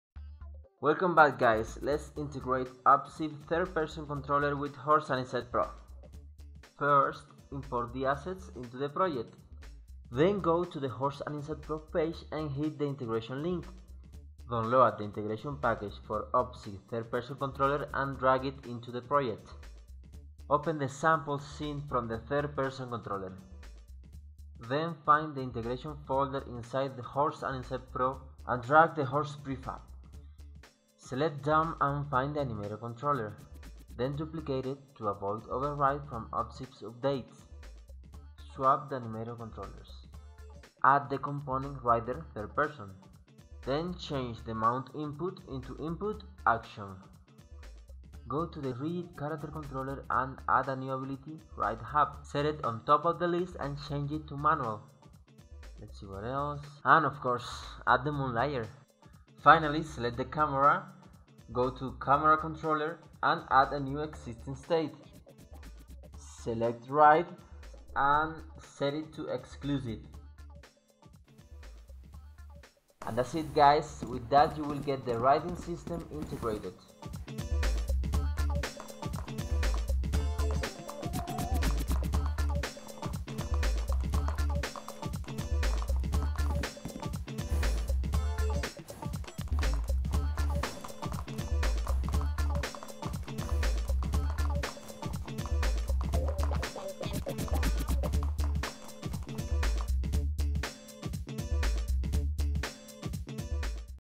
Opsive + HAP3 Part 1: Riding
These are the main steps for integrate the upcoming Horse Animset Pro 3 with Opsive Third Person Controller.